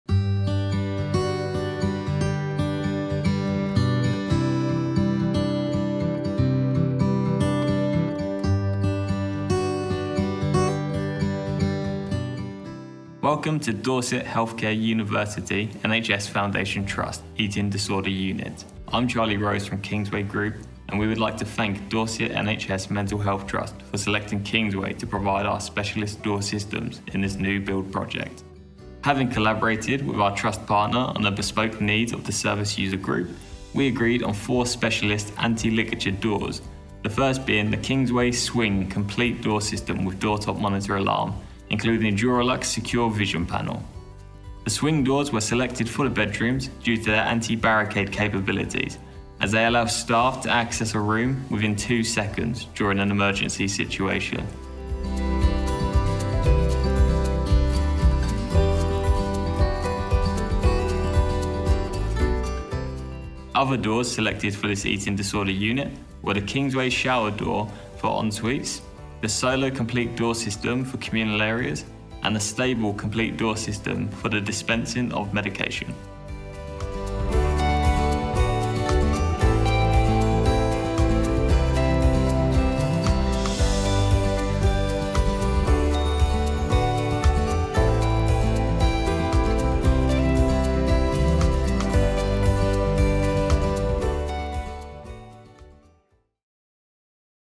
St Ann's Hospital, Eating Disorder Unit, Dorset HealthCare University NHS Foundation Trust
Today we welcome you to Dorset HealthCare University NHS Foundation Trust Eating Disorder Unit, at St Ann’s Hospital in Poole.

Having handed over on the completed 10-bedroom Kimmeridge Court ward earlier this month, Kingsway Group helped our NHS Trust partner successfully deliver a welcoming facility serving patients in the local area who, before the opening of the Kimmeridge Court, previously had to travel a much greater distance to receive the correct care.